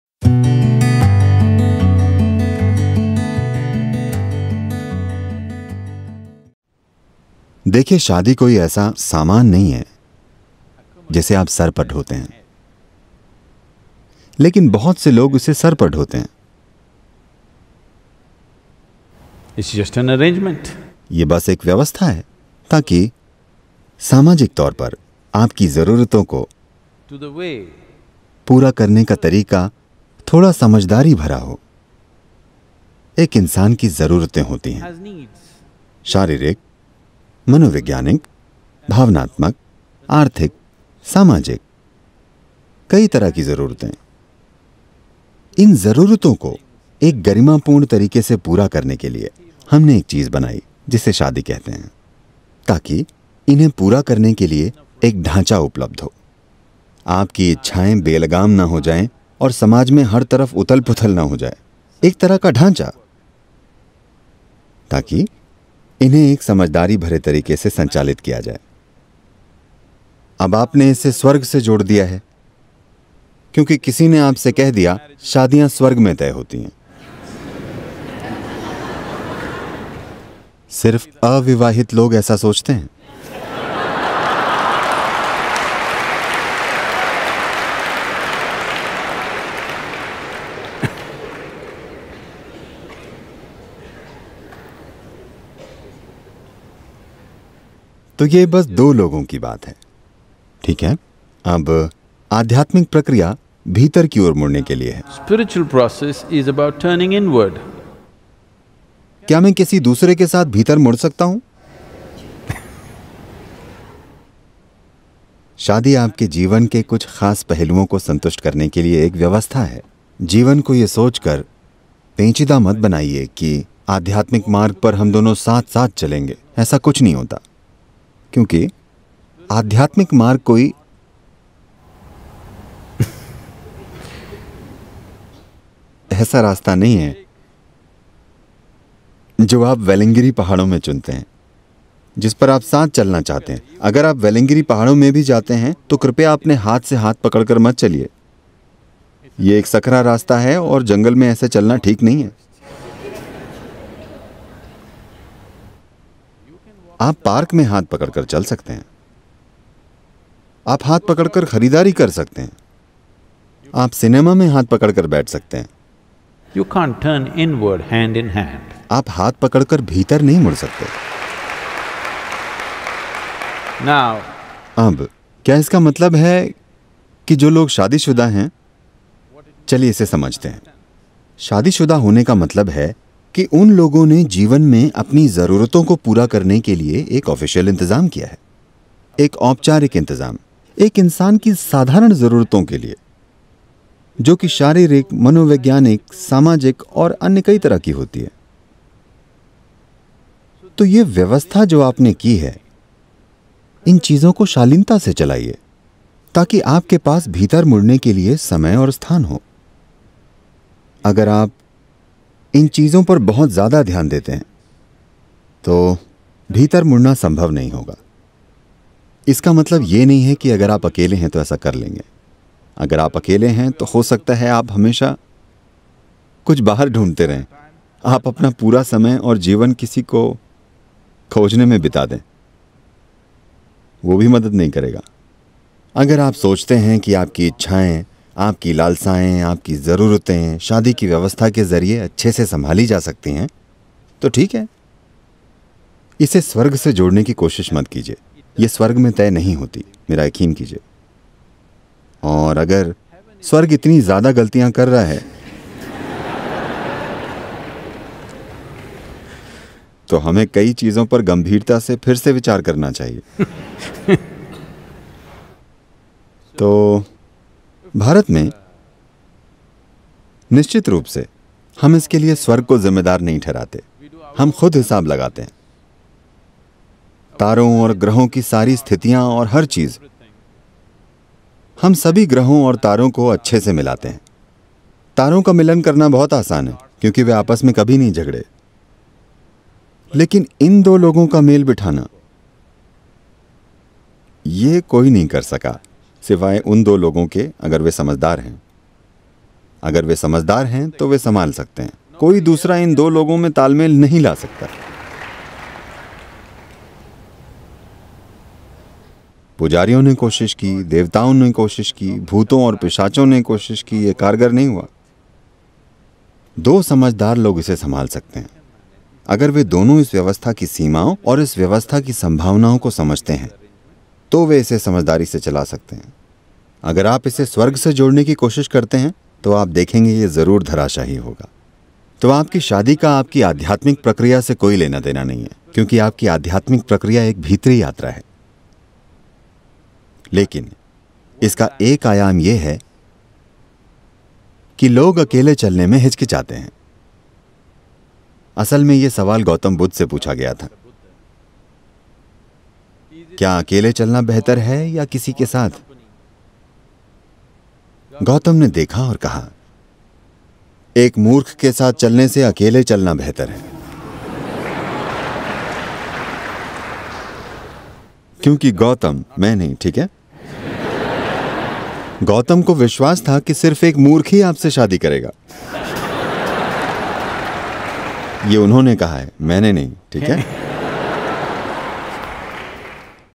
क्या शादी करना आध्यात्मिक राह में एक रुकावट है? | Sadhguru Hindi
सद्‌गुरु, शादी और आध्यात्मिक मार्ग से जुड़े एक प्रश्न का उत्तर दे रहे हैं। शादी एक ऐसी व्यवस्था है, जिसे हमने कुछ जरूरतें पूरी करने के लिए बनाया है। इसका अध्यात्म से कोई लेना देना नहीं है, क्योंकि आध्यात्मिक प्रक्रिया एक भीतरी यात्रा है। उम्मीद करते है की आप सब इस विडीओ को ध्यान से सुनेगे और अपने अपने विचार सोच समझ कर विवेक के साथ हमें कॉमेंट बॉक्स में भेजेंगे!